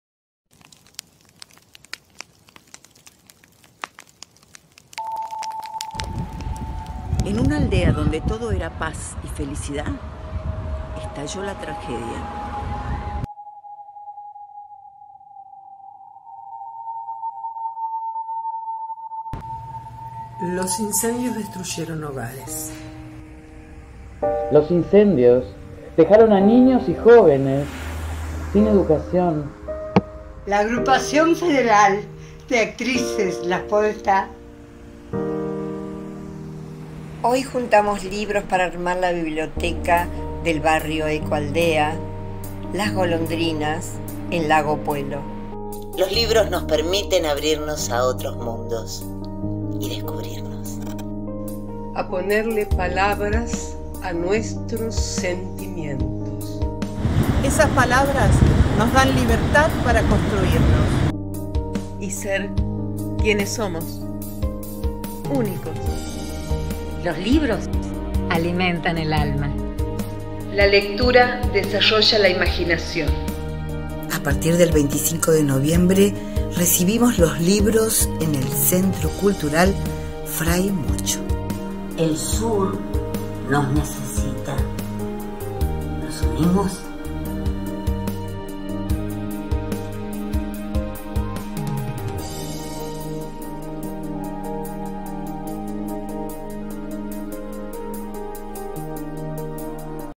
actrices argentinas..grupo de actrices. LAS PODESTÁ.... COLECTS SOLIDARIA...
un grupo de actrices argentinas de destacada trayectoria en las artes nacionales... suman está iniciativa... para que te sumes y puedas colaborar. en ella se encuentran..Patricia palmer- Graciela pal- Liliana simoni- Paola pappini y otras gdes y queridas actrices y artistas de la República Argentina.